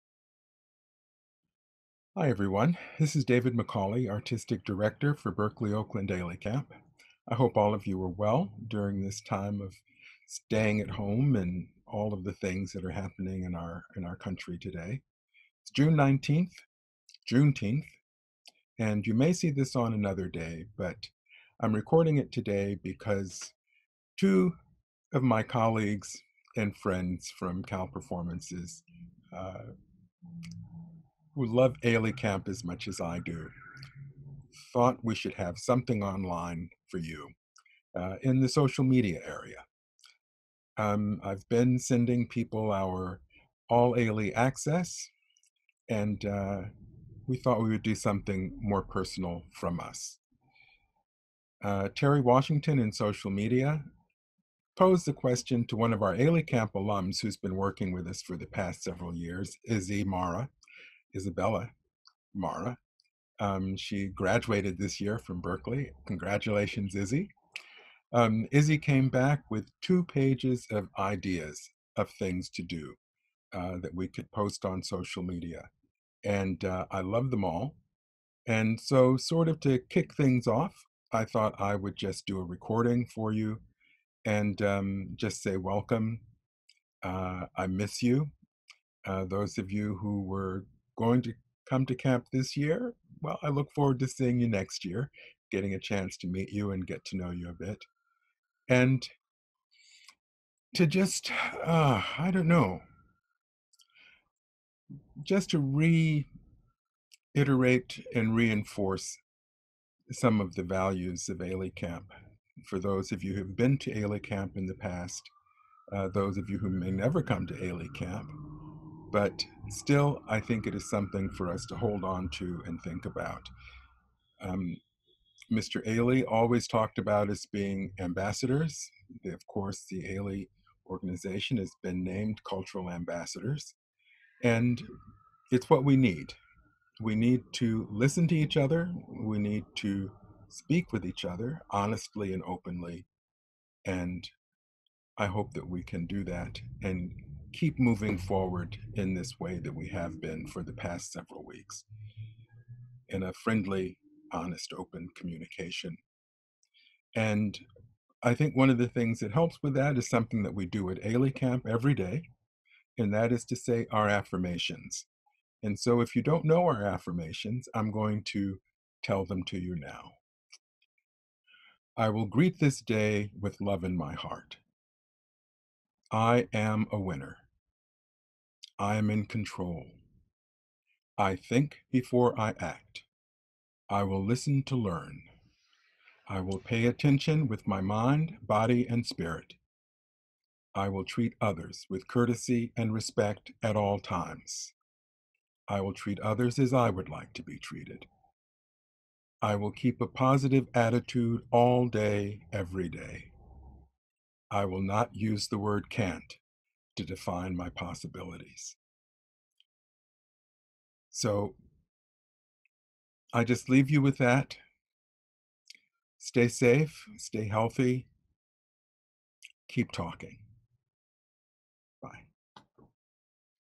AileyCamp 2020 Day 1
Today would've been the first day of AileyCamp for our campers. Our AileyCamp Artistic Director, David McCauley has a message for all of you.